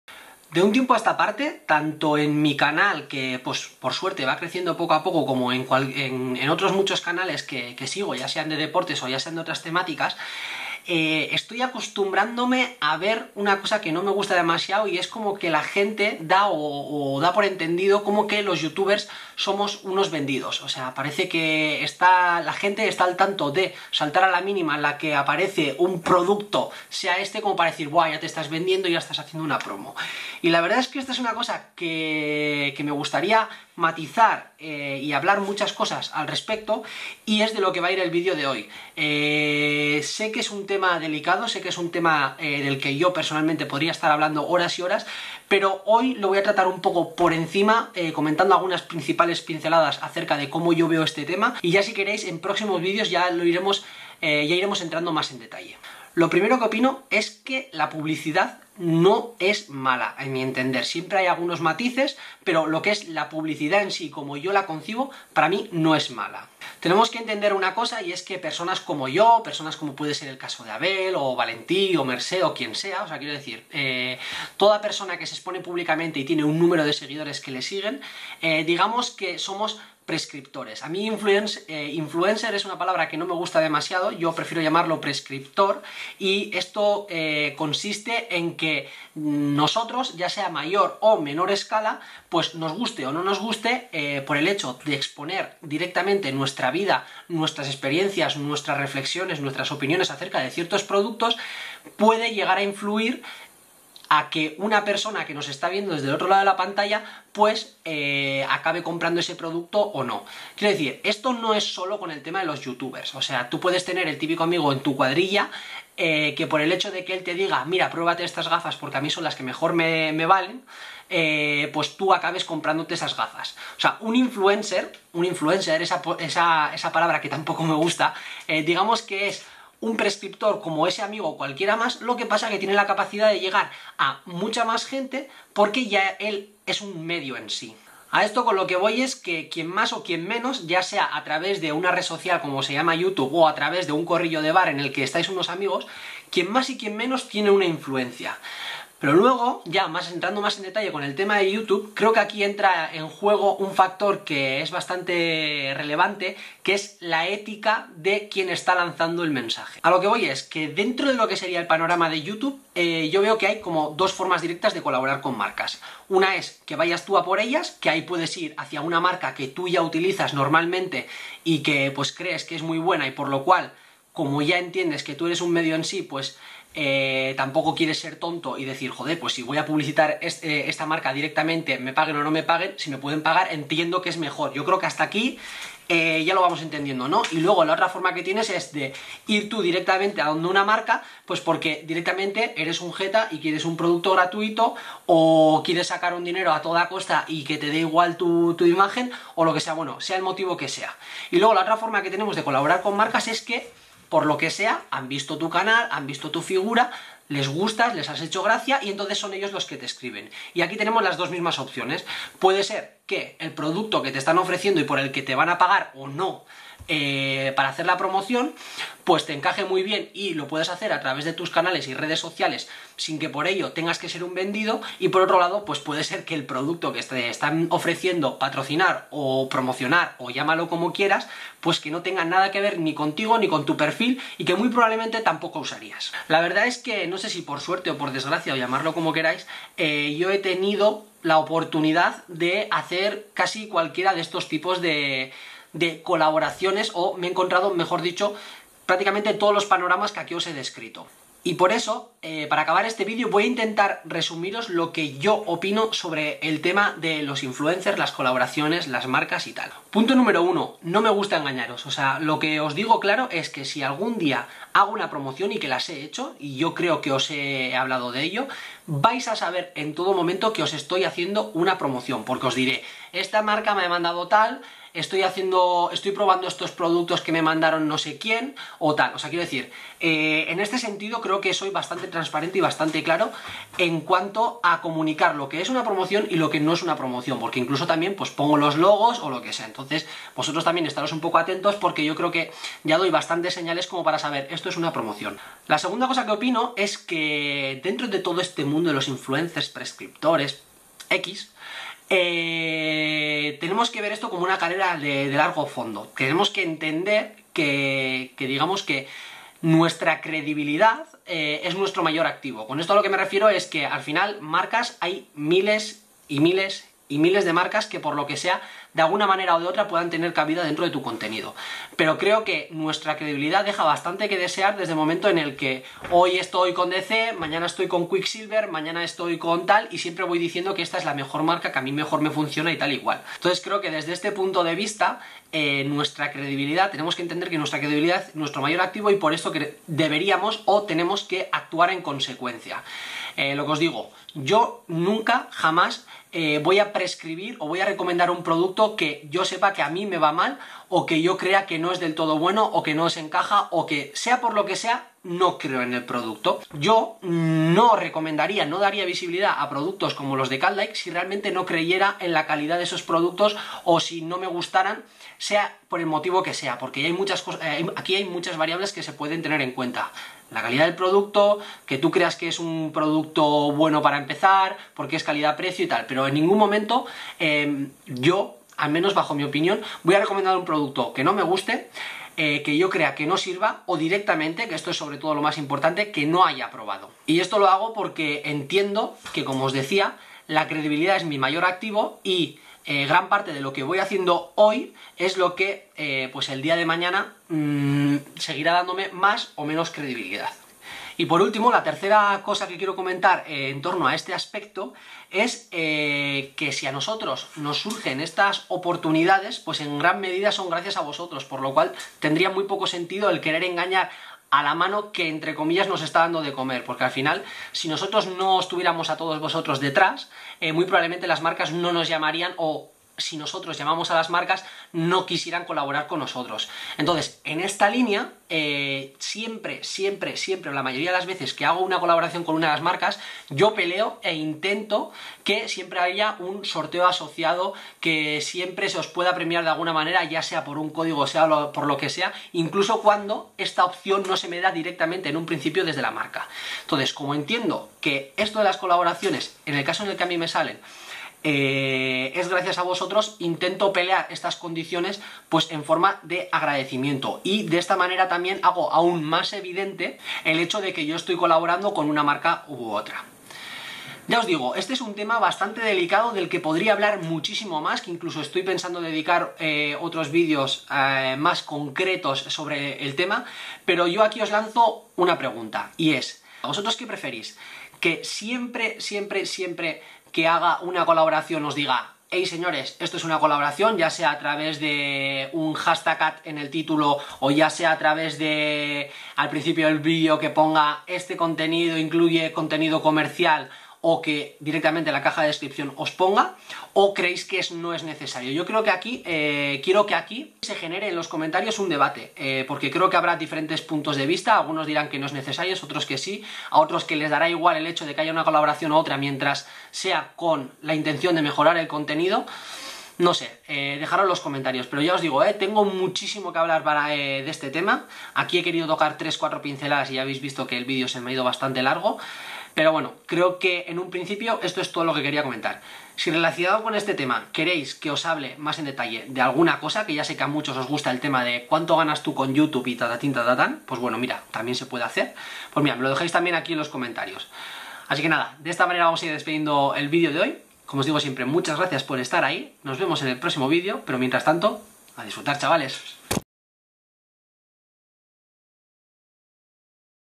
¿SOMOS LOS YOUTUBERS UNOS VENDIDOS?
Por lo que veo últimamente tanto en mis vídeos como en otros, parece que a la mínima que mostramos un producto y hablamos más o menos bien de él, la gente tiene la sensación de que estamos haciendo una promoción y que por ello somos unos vendidos. Pero no es así. Por eso, en este vídeo intento asincerarme al respecto y mostrar mi punto de vista sobre el tema para dejar claras algunas cosas.

Si os surge alguna duda o no he dejado del todo claro algunos de los temas que aquí trato, no tenéis más que dejármelo en los comentarios que os intentaré responder a todos.

Por otro lado, me gustaría que me dijerais qué otros temas relacionados a éste os gustaría que tratara y sobre todo que debatáis sobre si debemos o no indicar en todo momento lo que es una promoción y lo que no...

Lo dejo en vuestras manos ;)